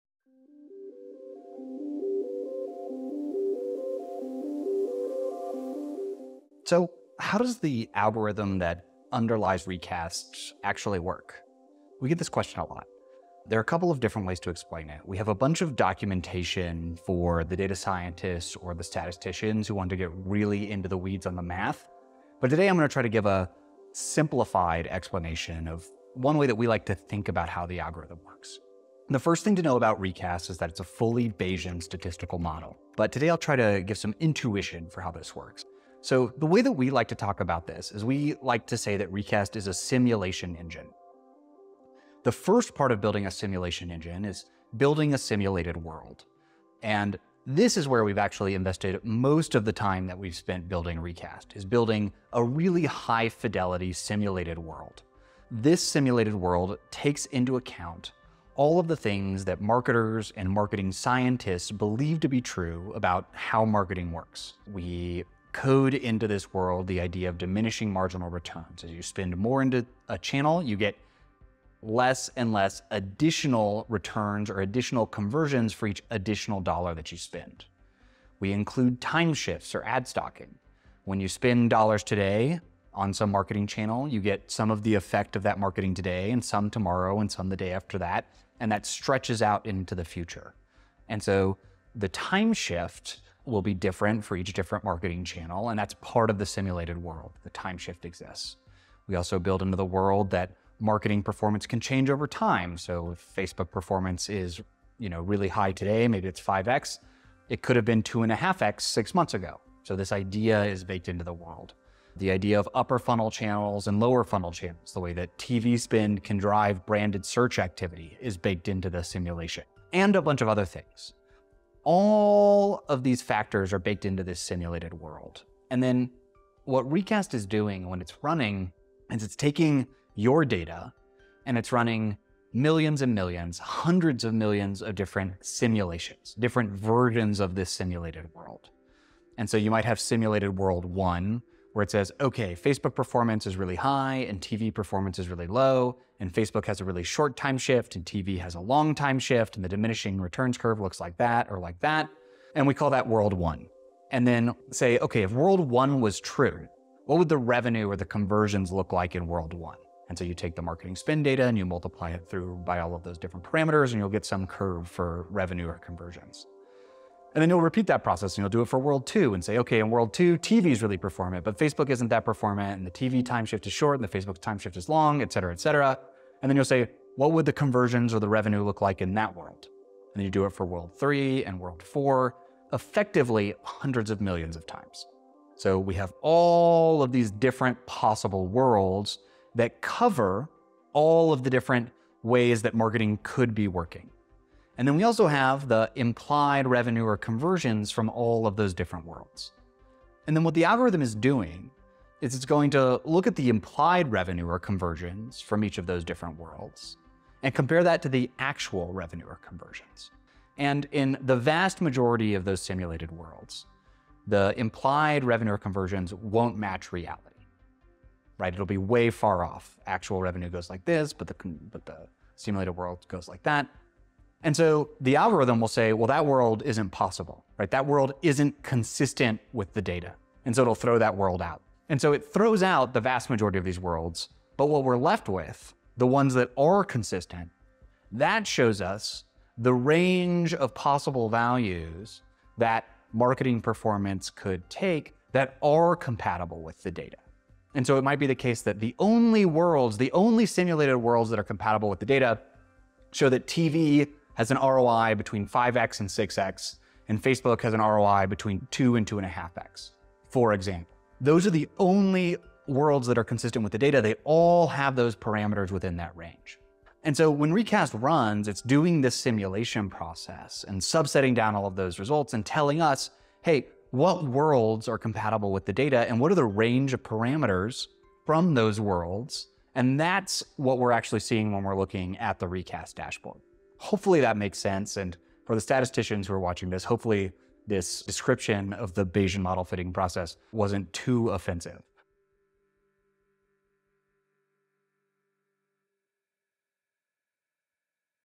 Bayesian MMM explained: The algorithm behind Recast
How does the algorithm that underlies Recast actually work? We get this question a lot, and there are a couple of different ways to explain it.

In this video, our co-founder Michael Kaminsky gives a simplified explanation of one way that we like to think about how the algorithm works.

We have a bunch of documentation for data scientists or statisticians who want to get really into the weeds on the math which you can also check